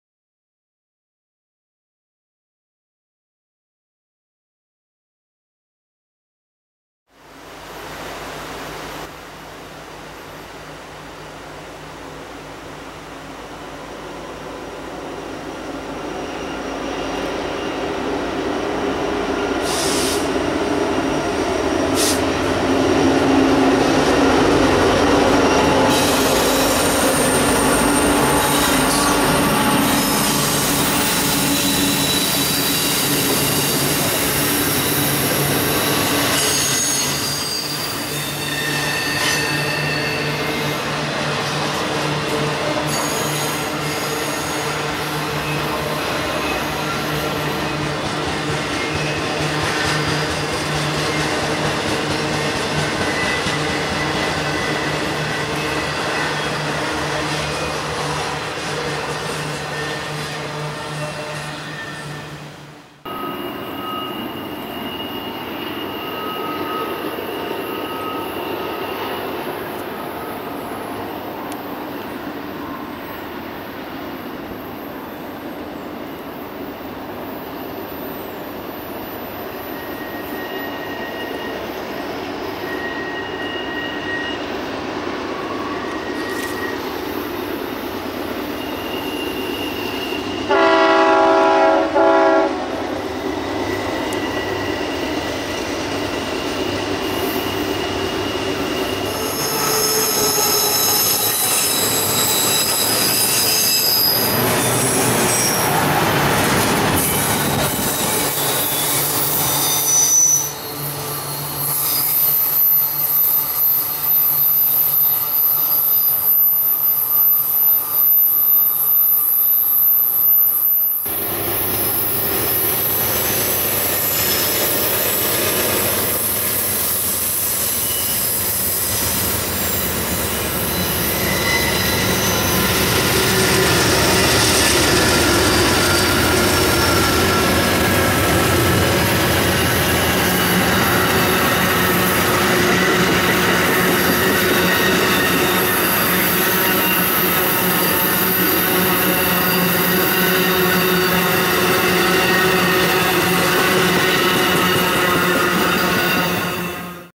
Troy/Lower Cascade Bridge July 3 2015
Showed a friend this area for a few hours.  Managed to nab 3 trains before the afternoon heat kicked in.